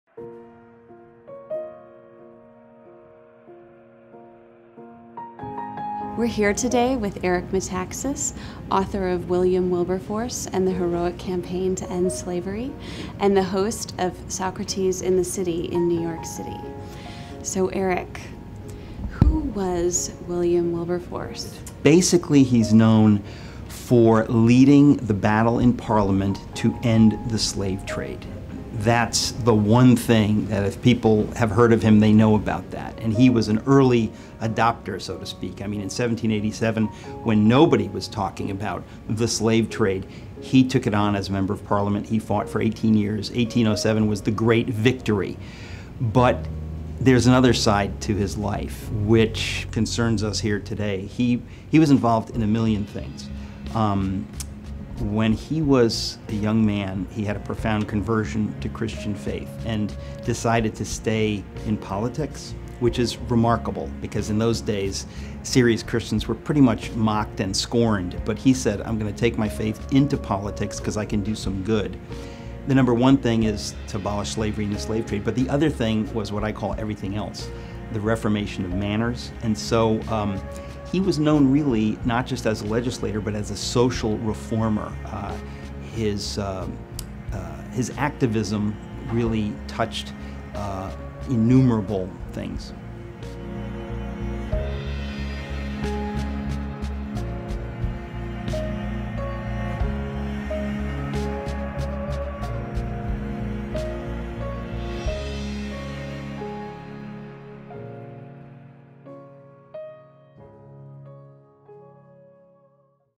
Eric Metaxas: William Wilberforce & Animals Part 1 of 4
In this series, three modern-day faith leaders discuss three historical figures whose contributions to animal welfare stemmed directly from their religious convictions.

Eric Metaxas, author of Amazing Grace and host of New York City’s, Socrates in the City, begins the series with a discussion of William Wilberforce, an early-19th century English Parliamentarian best known for his heroic efforts to end slavery. “For Wilberforce, animal activism came out of his faith,”says Metaxas. “He said it was wrong and cruel and ultimately something that courses human beings.”